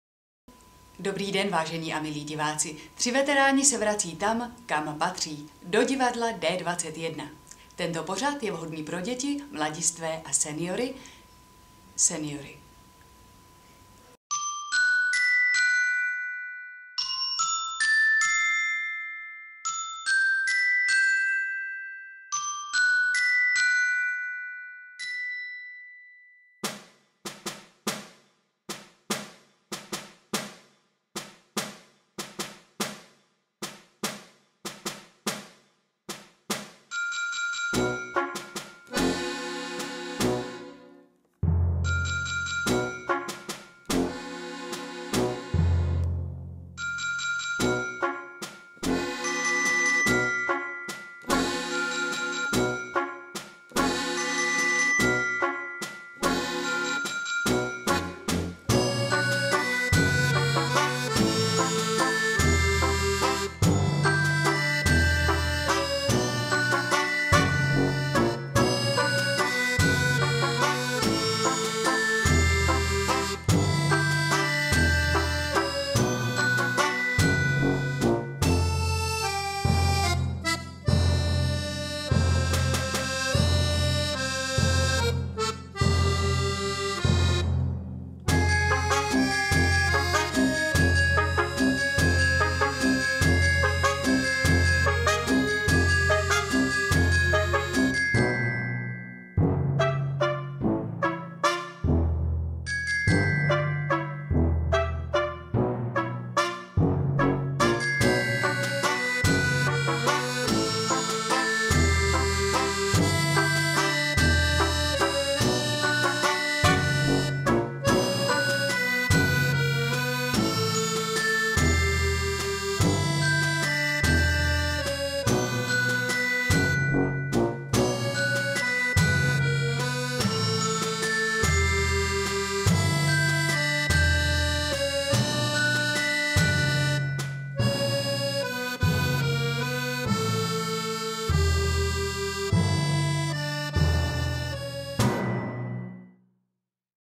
Divadlo D21: 3 Veteráni -- trailer
Divadlo D21 uvádí inscenaci slavné Werichovy pohádky 3 veteráni.
Hrají: Marie Hodinářová, Stela Chmelová, Hana Mathauserová
Režie: Jiří Ondra.
Pro D21 vyrobil V.O.K.R. Kamera: Aleš Němec. Střih a režie: Peter Chmela & Elie Razzouk